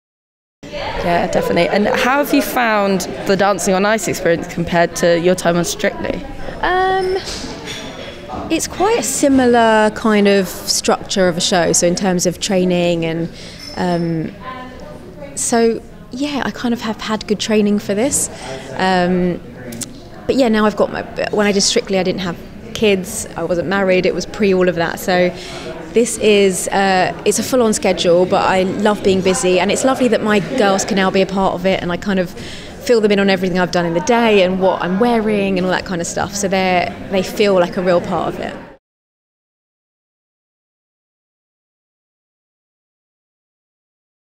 Rachel Stevens on how Dancing on Ice compares to Strictly
Dancing on Ice 2022 contestant Rachel Stevens reveals how the ITV competition compares to hitting the Strictly dancefloor in an exclusive interview with RadioTimes.com.